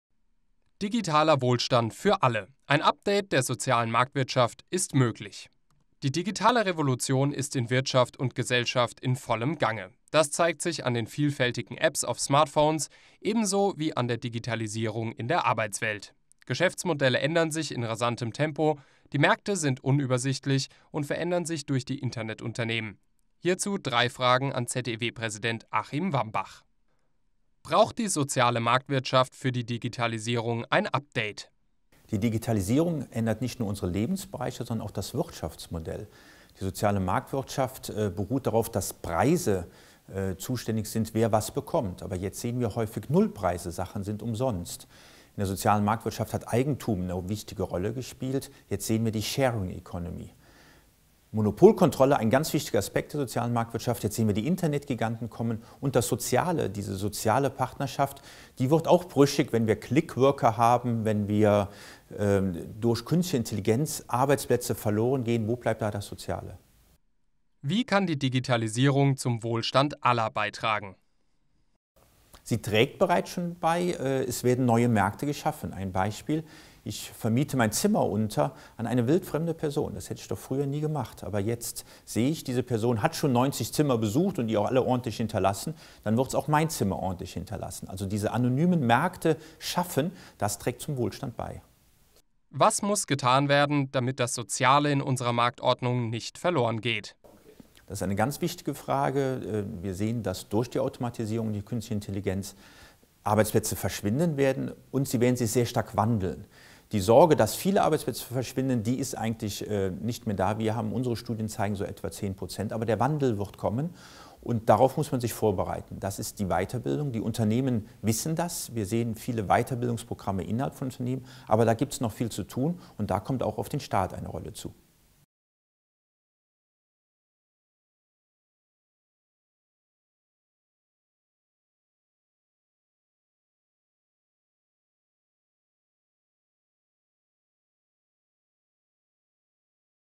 Digitaler Wohlstand für alle: Ein Update der Sozialen Marktwirtschaft ist möglich
Die digitale Revolution ist in Wirtschaft und Gesellschaft in vollem Gange. Das zeigt sich an den vielfältigen Apps auf Smartphones ebenso wie an der Digitalisierung in der Arbeitswelt. Geschäftsmodelle ändern sich in rasantem Tempo, die Märkte sind unübersichtlich und verändern sich durch die Internetunternehmen. Hierzu drei Fragen an ZEW-Präsident Achim Wambach…